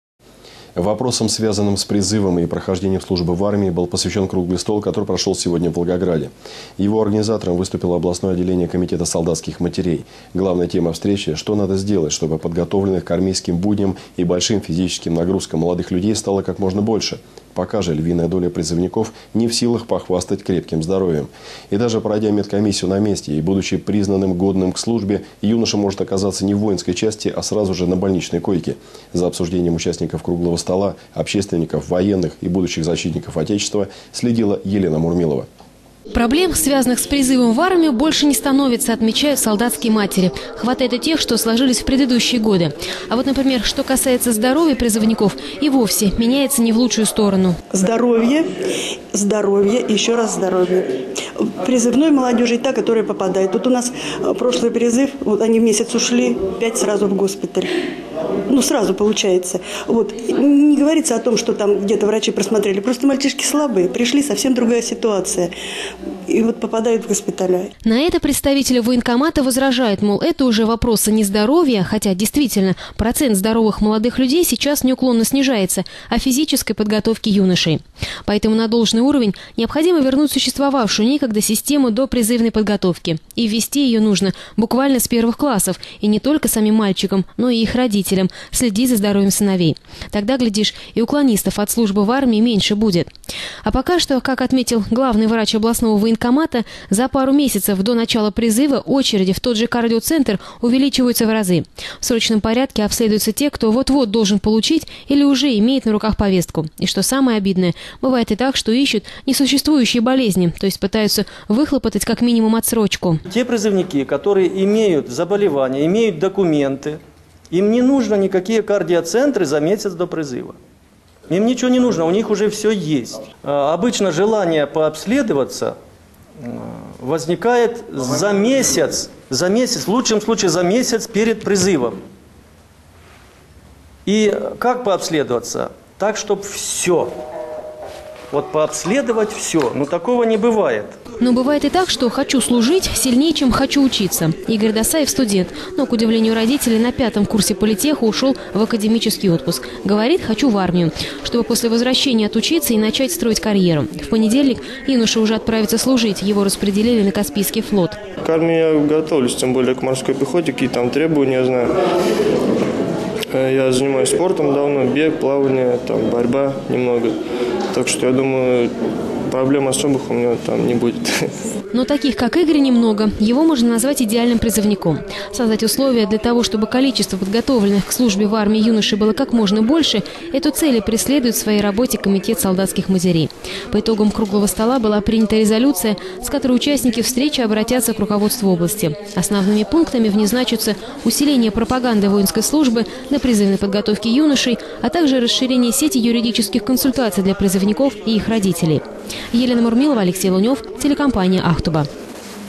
ПРОЦЕНТ ЗДОРОВЫХ МОЛОДЫХ ЛЮДЕЙ НЕУКЛОННО СНИЖАЕТСЯ
Вопросам, связанным с призывом и прохождением службы в армии, был посвящен круглый стол, который прошел сегодня в Волгограде. Его организатором выступило областное отделение Комитета солдатских матерей. Главная тема встречи - что надо сделать, чтобы подготовленных к армейским будням и большим физическим нагрузкам молодых людей стало как можно больше? Пока же львиная доля призывников не в силах похвастать крепким здоровьем. И даже пройдя медкомиссию на месте и будучи признанным годным к службе, юноша может оказаться не в воинской части, а сразу же на больничной койке.